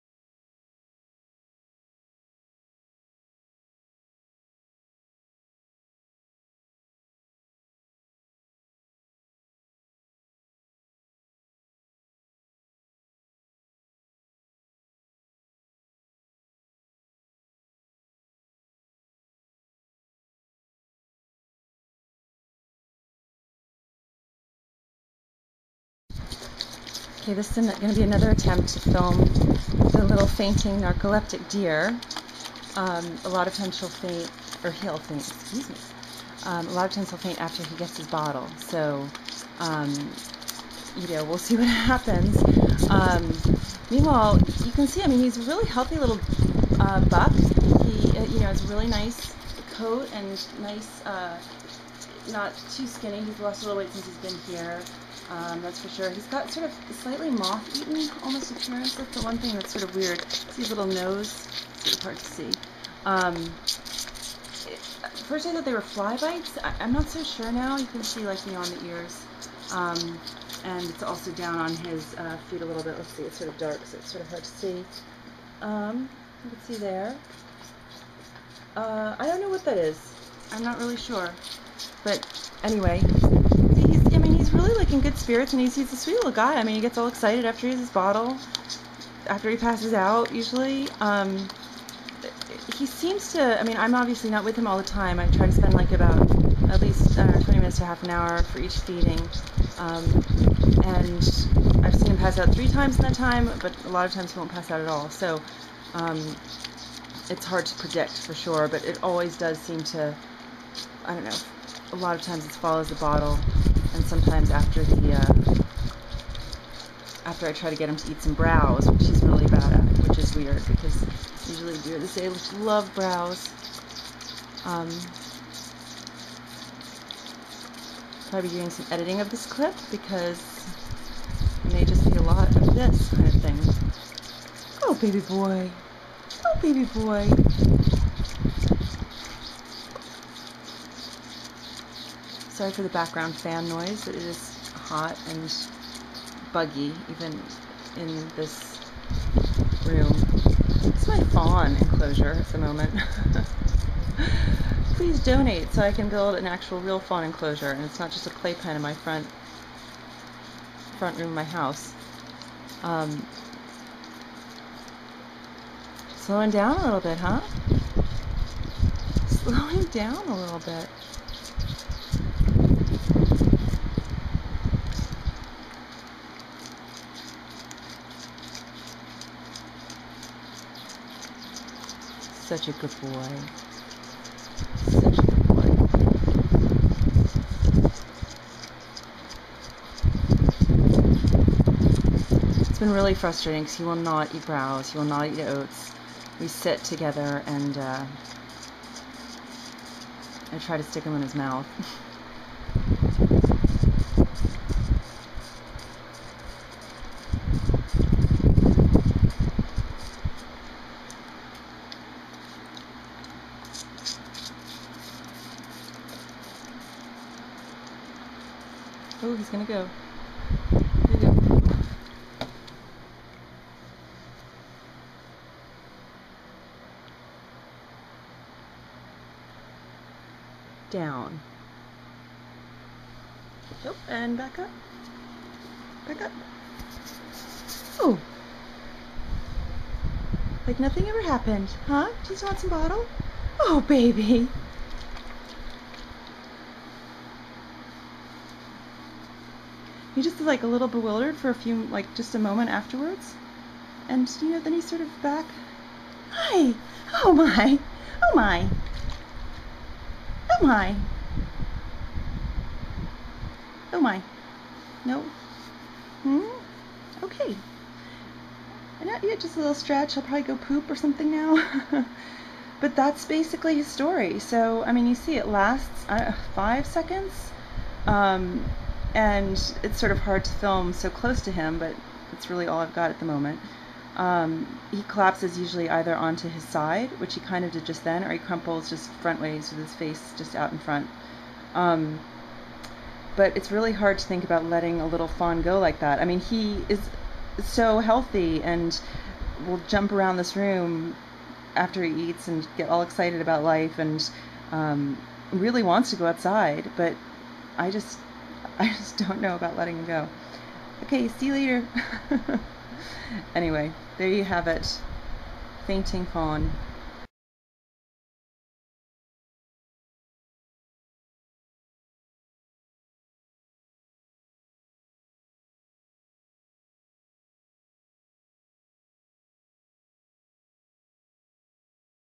The Fainting Fawn 7-17-11.wmv
"Victor" is a ~5 week old White Tailed Deer fawn who arrived at Wild Things Sanctuary July 13, 2011. He had some odd looking scratches and scrapes, but in general was in a good body condition. We decided to observe him for a few days and then work on slowly introducing him to the other fawns, now outdoors and free roaming, at Wild Things Sanctuary.

...but within an hour of his arrival, he fainted!

There are no obvious signs of injury, trauma, nutritional deficiency, weakness, cognitive impairment, parasites, etc. that one would assume would accompany animals who faint. We have been treating him for nutritional issues, dewormed him, etc. but have not see any obvious difference. Could be a possible issue with the Vagus nerve that mediates a lot of heart and lung functions.

Someone also suggested NARCOLEPSY. After watching some videos it does appear that he faints very much like narcoleptic dogs, though this behavior has never been observed in deer.

The verdict is still out as to what is wrong with Victor. We hope that fainting is something that he will outgrow and we are building him a lovely outdoor enclosure at the moment so that we can observe him closely.

Behaviorally Victor is a little too tame for a fawn his age, this and other details make me think that he may have been raised by a person who let him go when he started having his fainting spells. By keeping him outdoors with other fawns and away from people we hope that he can "wild up" and be ready for release if and when he is ready.

The fainting spells don't seem to bother him at all...but it is unclear whether they will get worse. It is also not the most ideal behavior for a prey animal!

I'm hoping to get some better video footage, but this is what I have at the moment. I thought it would be useful to get it out there in case others can illuminate his condition.

VICTOR UPDATE 3/1/12: I am super happy to report that Victor is no longer fainting! After reaching out to other wildlife rehabilitators all over the US, I discovered that there have been a few cases of fainting fawns. They end up being unreleasable as their human caregivers keep them close to watch over them, and then the fawns end up being too socialized.

So, Victor was sent to a wonderful rehab center specializing in fawn care and was able to be closely monitored, but surrounded by about 40 other fawns his own age as well as a few adult White Tailed Deer. In this way, he did NOT become over socialized with humans and will be released in the coming months.

YAY for Victor and all the people who helped him grow up healthy!!!